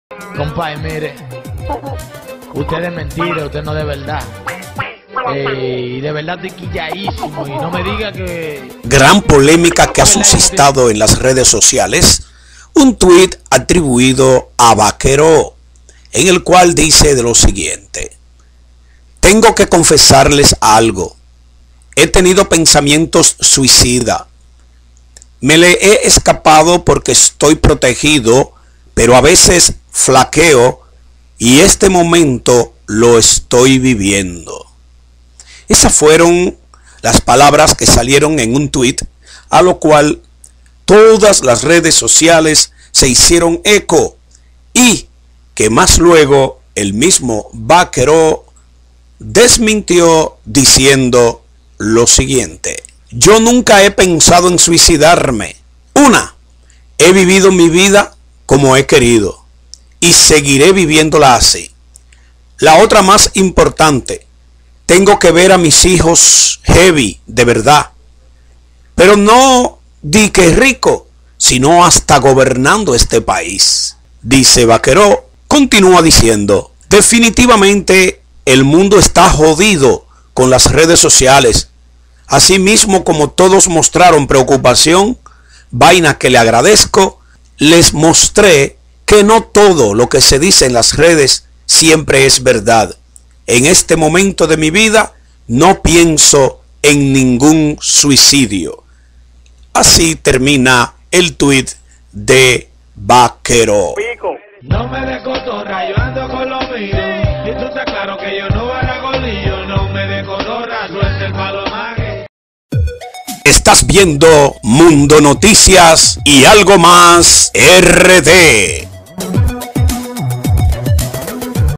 EL SUICIDIO DE VAKERO
TENGO QUE CONFESARLE  ALGO, he tenido pensamientos suicida,me le he escapado porque estoy protegido pero a veses flaqueo y este momento lo estoy viviendo, asi incio una gran polemica sobre lo que dice vakero, pero vakero dice en otro twitt que nunca a pensado el  suicidio y que las personas estan mal, por llevar todo a las redes sociales y creerles